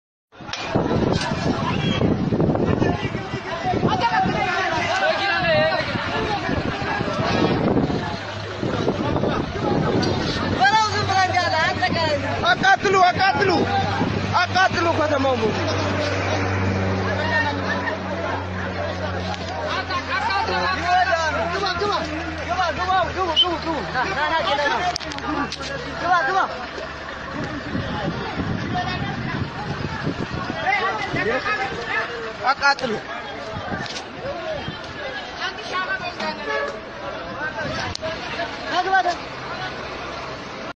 Ethiopian national defense forces film themselves torching an entire town in Tigrai
Ethiopian national defense forces film themselves torching an entire town in Tigrai as residents flee abandoning their homes.
Soldiers say "blow everything indiscriminately", "burn it, burn the whole town!"

Subtitled by አባ አነንያ

ENDF soldier's Tiktok